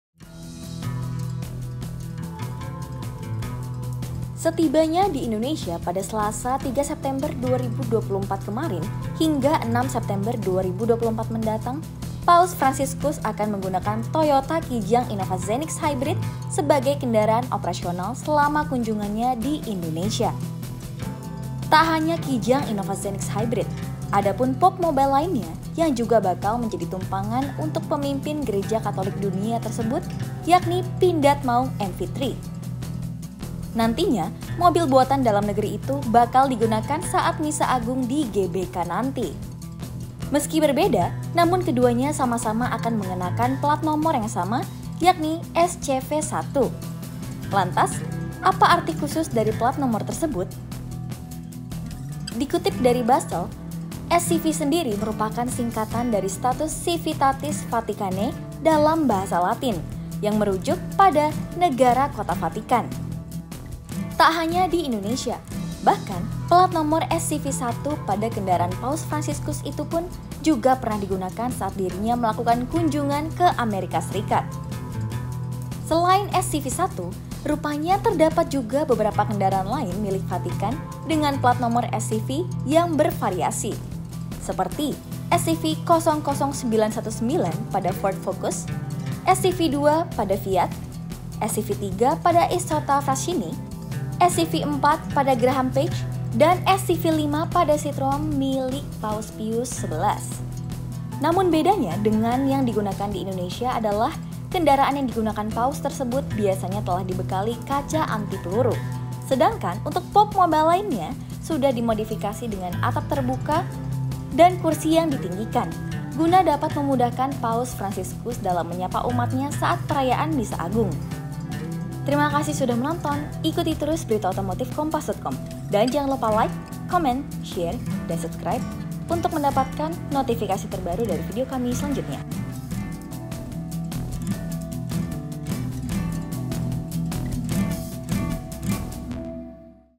Mobil Paus Fransiskus Pakai Pelat Nomor SCV 1, Apa Artinya?
Tak hanya Kijang Innova Zenix Hybrid, ada pun popemobile lainnya yang juga bakal menjadi tunggangan untuk Pemimpin Gereja Katolik Dunia tersebut yakni Pindad Maung MV3. Nantinya, mobil buatan dalam negeri itu bakal digunakan saat Misa Agung di GBK nanti. Meski berbeda, namun keduanya sama-sama akan mengenakan pelat nomor yang sama, yakni SCV 1. Lantas, apa arti khusus dari pelat nomor tersebut?

Jika anda menyukai video seperti ini bisa tonton video :
Kompas.com
Video Editor : Carolus Dori. K
Penulis Naskah : Carolus Dori. K
Pengisi Suara : Maria Rarindra